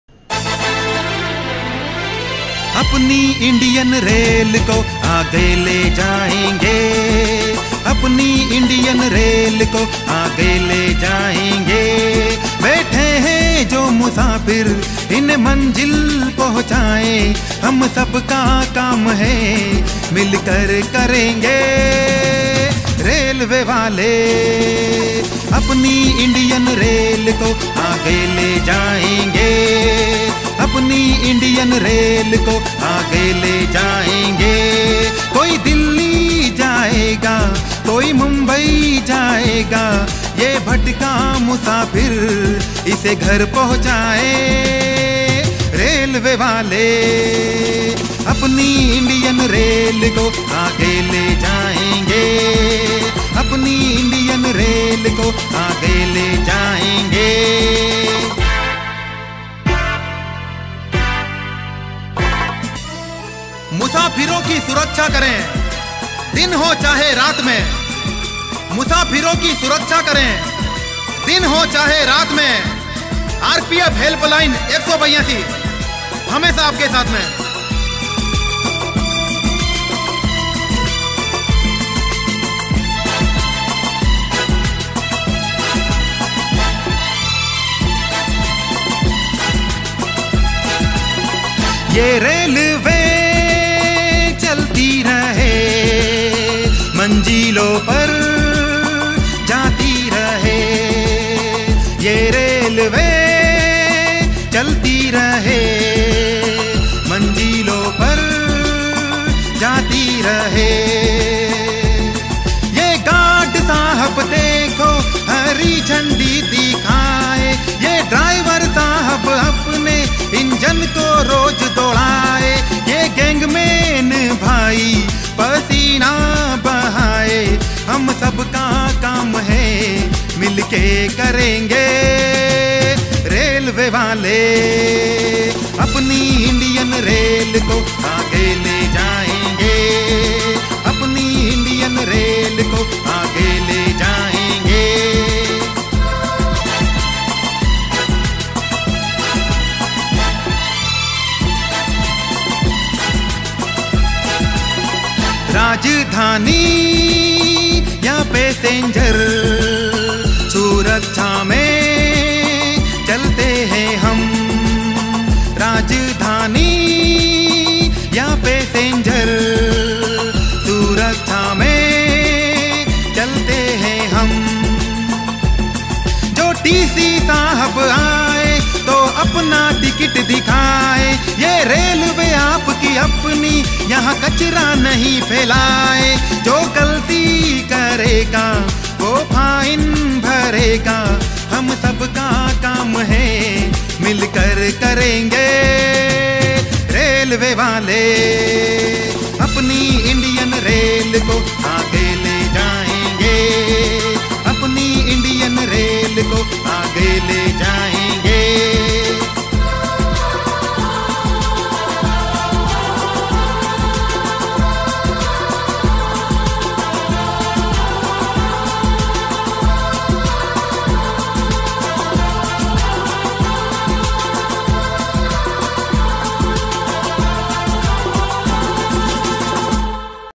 Indian Railway ke sthapna Divas per Shandar Indian Railway song
Bhartiya Railway ke 166 Varsha pure hone per yah Shandar Railway song